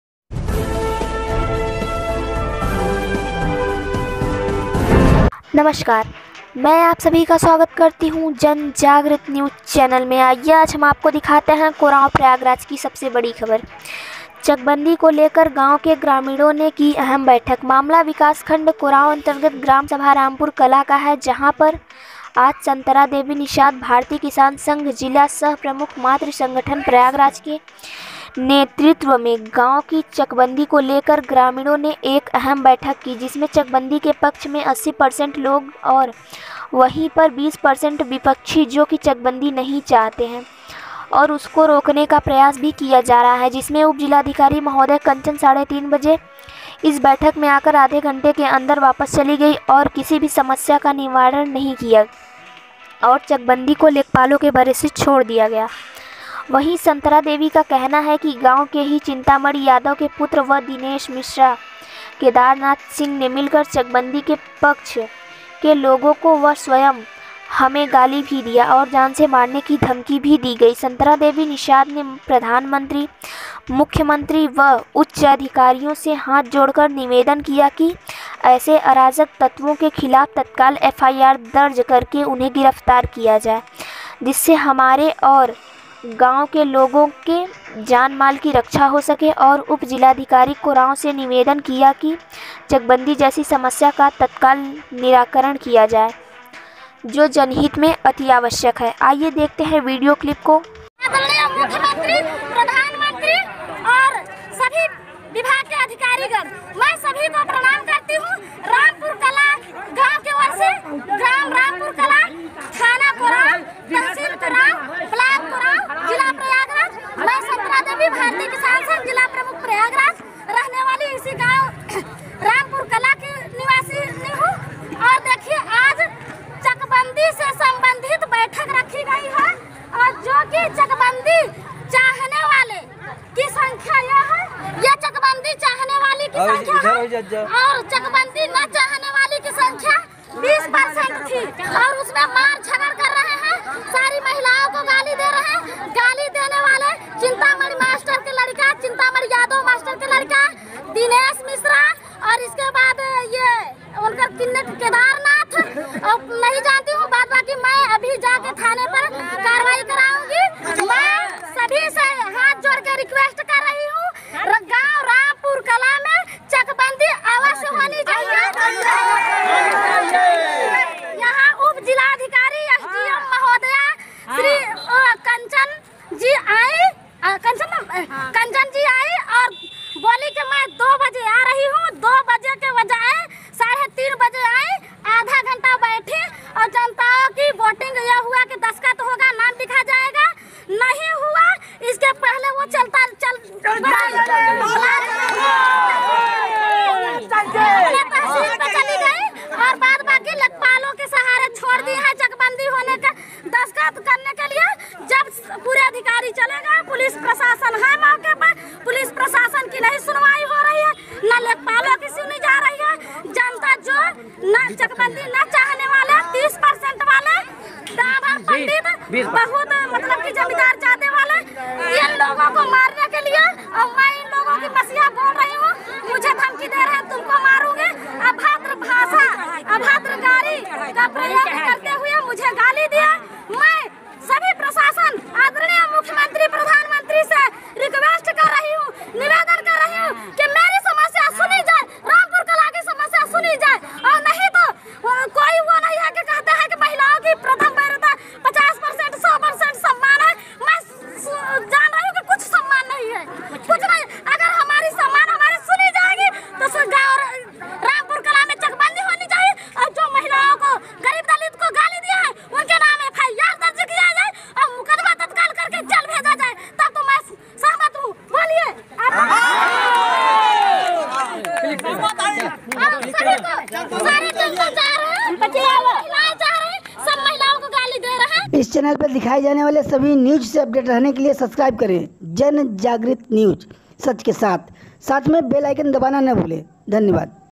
ग्राम सभा रामपुर कला मे संतरा देवी निषाद के नेतृत्व में चकबंदी को लेकर ग्रामीणों ने एक बैठक।Ep-649।
कोरांव प्रयागराज
 चकबंदी को लेकर गांव के ग्रामीणों ने की अहम बैठक मामला विकासखंड कोरांव अंतर्गत ग्राम सभा रामपुर कला का है जहां पर आज संतरा देवी निषाद भारतीय किसान संघ जिला सह प्रमुख मातृ संगठन प्रयागराज के नेतृत्व में गांव की चकबंदी को लेकर ग्रामीणों ने एक अहम बैठक की जिसमें चकबंदी के पक्ष में 80% लोग थे और वहीं पर 20% विपक्षी जो की चकबंदी नहीं चाहते हैं और उसको रोकने का प्रयास भी किया जा रहा है जिसमें उप जिलाधिकारी महोदय कंचन 3:30 बजे इस बैठक में आकर आधे घंटे के अंदर वापस चली गई और किसी भी समस्या का निपटारा नहीं किया और चकबंदी को लेखपालों के भरोसे छोड़ दिया गया । वहीं संतरा देवी का कहना है कि गांव के ही चिंतामणि यादव के पुत्र व दिनेश मिश्रा, केदारनाथ आदि ने मिलकर चकबंदी के पक्ष के लोगों को व स्वयं हमें गाली भी दिया और जान से मारने की धमकी भी दी गई ।संतरा देवी निषाद ने प्रधानमंत्री, मुख्यमंत्री व उच्च अधिकारियों से हाथ जोड़कर निवेदन किया है कि ऐसे अराजक तत्वों के खिलाफ तत्काल एफ आई आर दर्ज करके उन्हें गिरफ्तार किया जाए जिससे हमारे और गांव के लोगों के जानमाल की रक्षा हो सके और उप जिलाधिकारी कोरांव से निवेदन किया है कि चकबंदी जैसी समस्या का तत्काल निराकरण किया जाए जो जनहित में अति आवश्यक है ।
आइए देखते हैं वीडियो क्लिप को और जानते हैं सच्चाई क्या है